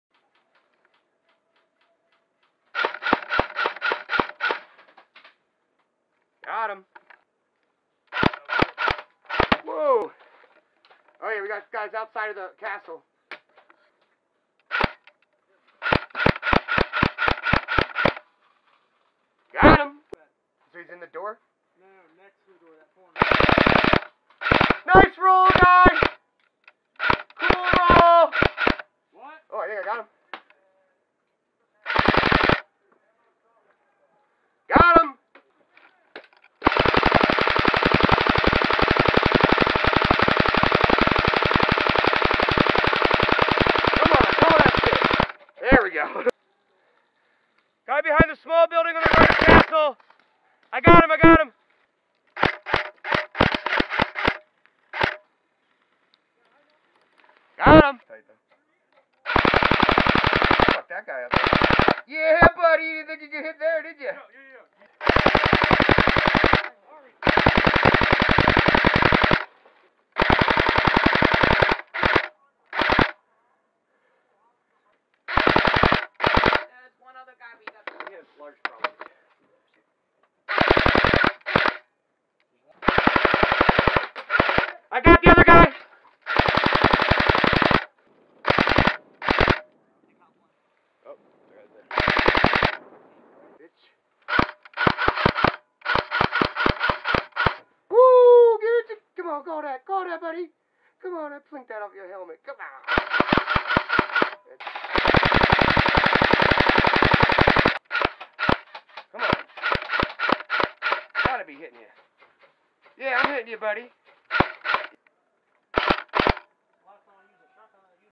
Airsoft: Gun Cam Kills
got a good spot held up in a camper just wait for them to walk into my killzone. ambush airsoft PA.